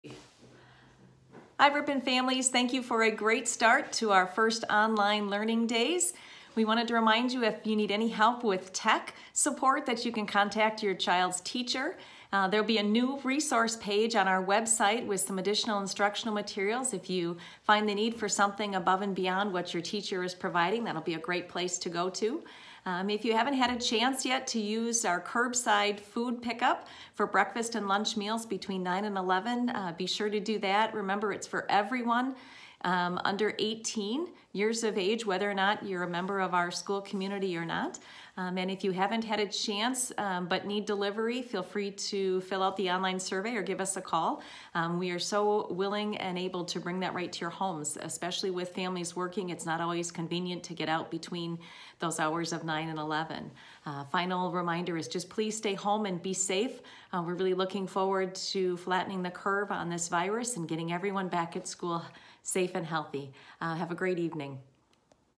March 26, 2020 Update From Dr. Mary Whitrock
RASD Superintendent Dr. Mary Whitrock provides an update to community members in this video